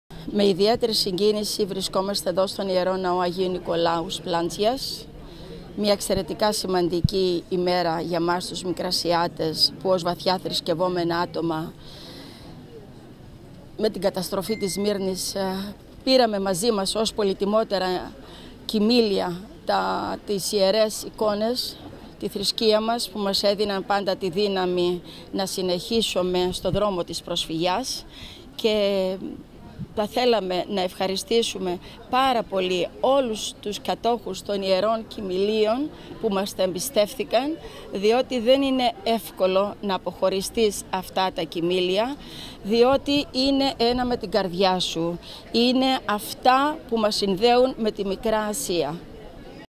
Πρόεδρος της Αδελφότητας Μικρασιατών Ν. Χανίων «Άγιος Πολύκαρπος» Στέλλα Γκοζάνη – Χαριτάκη
Έκθεση κειμηλίων στον Ναό του Αγίου Νικολάου Σπλάντζιας από μικρασιάτες